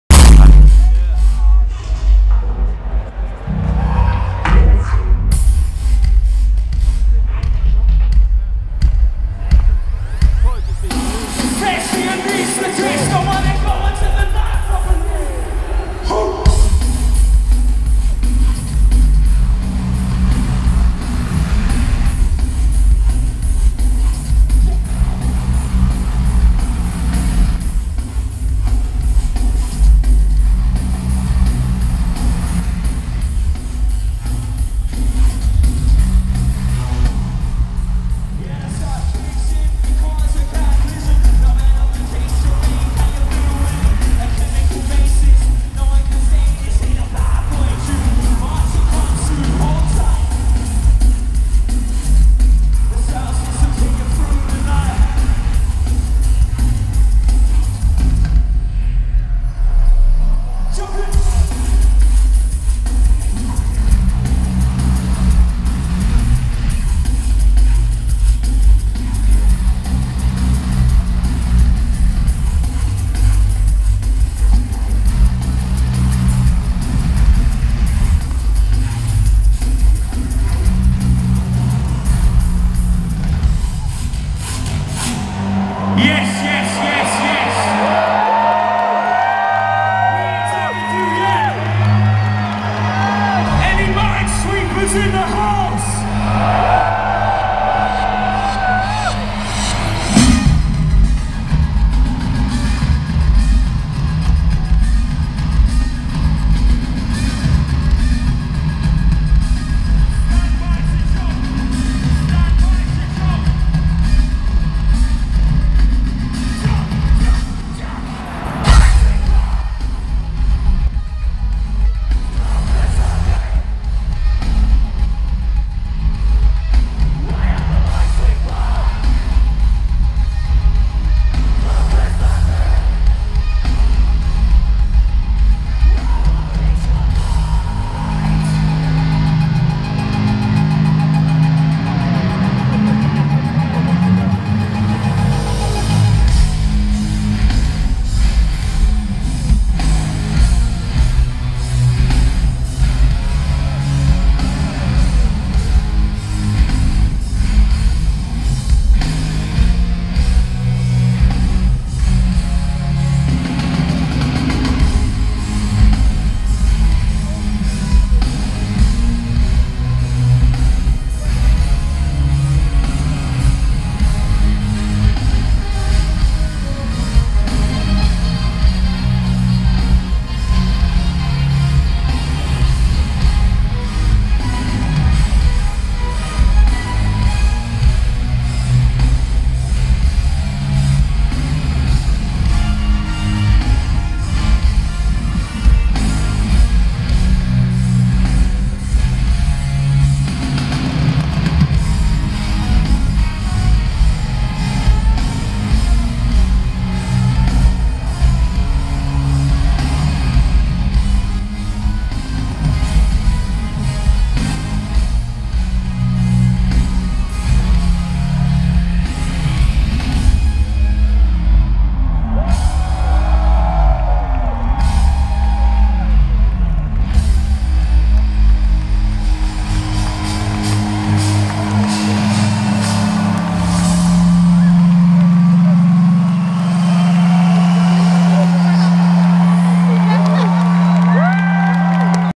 Enter Shikari Medley Live at Ally Pally 28 2 16
Enter Shikari playing a medley live at Ally Pally 28.2.16